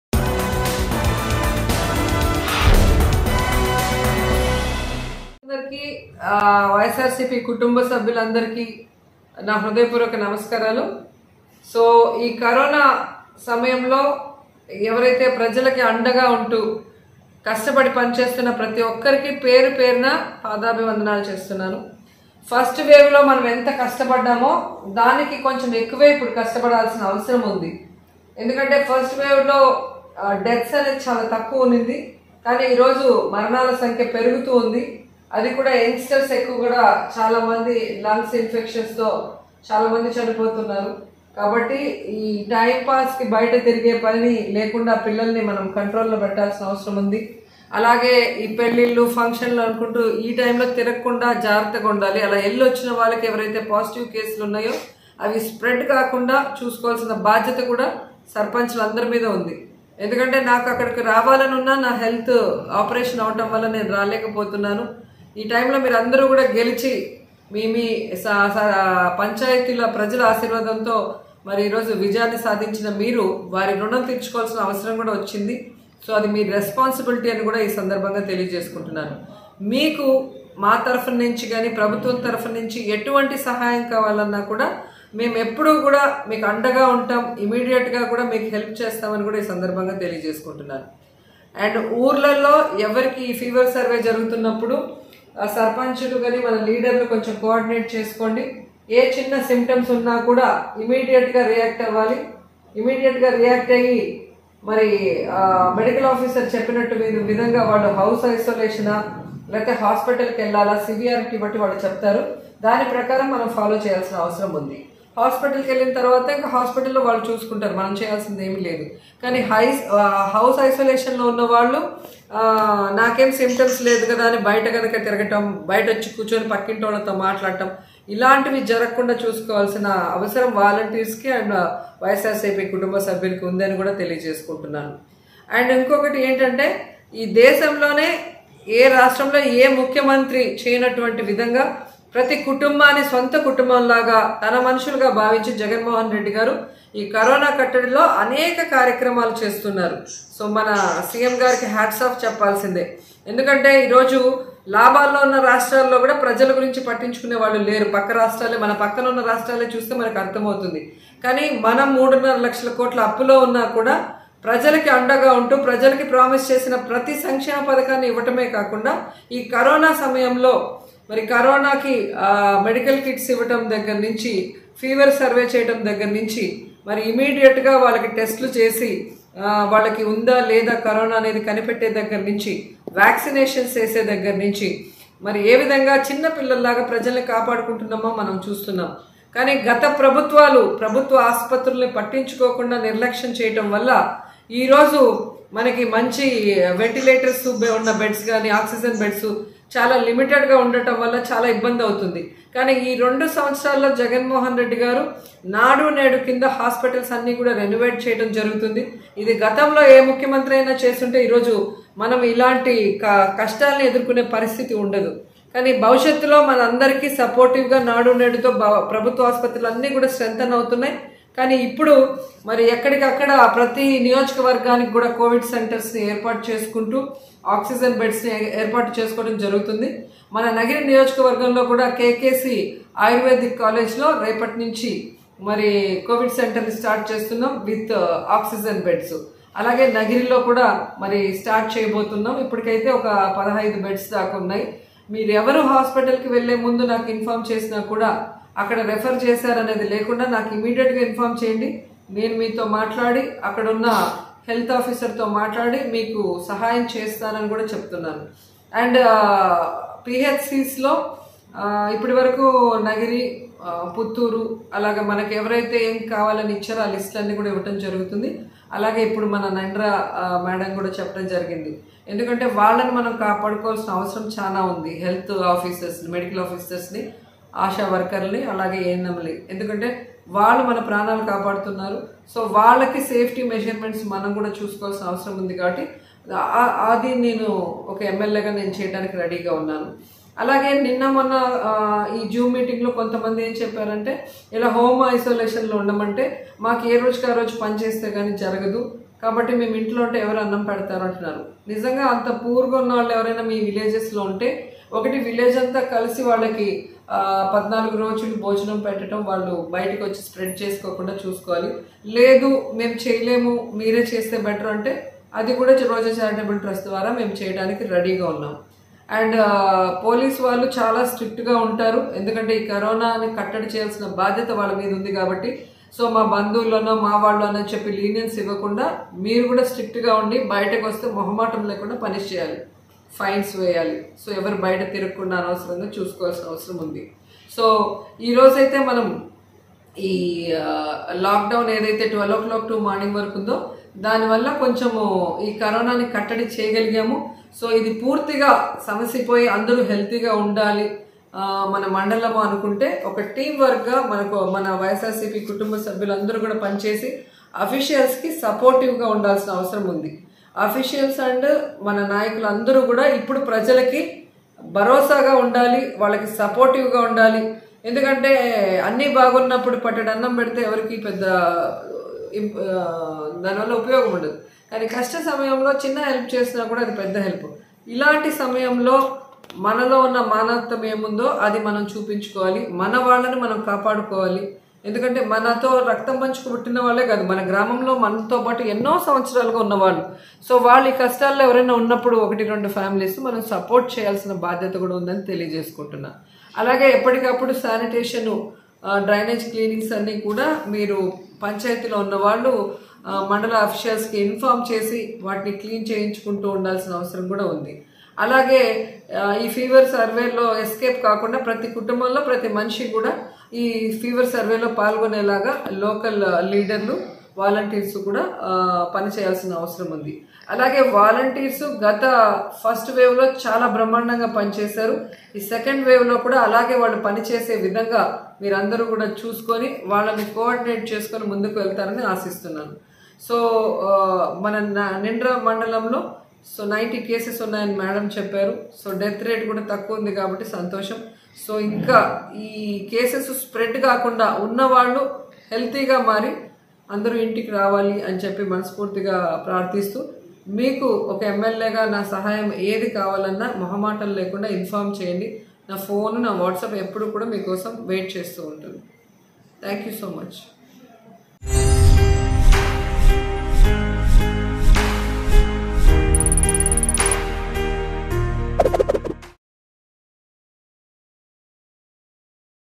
YCP MLA Roja Great Words About CM YS Jagan Over His Ruling | AP Political Updates | Mango News
YCP MLA Roja Great Words About CM YS Jagan Over His Ruling.